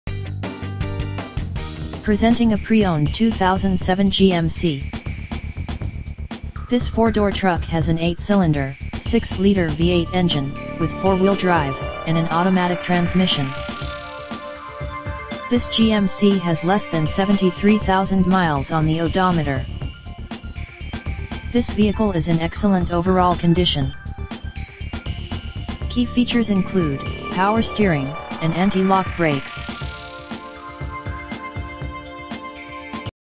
2007 GMC Sierra 2500HD Classic SLE 4x4 for sale in Lakewood, NJ
2007 GMC SIERRA SLE 2500 HD HEAVY DUTY 4X4 EXCAB, CLOTH, POWER WINDOWS, AM/FM CD, POWER SEAT, BEDLINER, TOW PKG., 6 1/2FT BED, ALLOY WHEELS, CLEAN CARFAX HISTORY, WE FINANCE!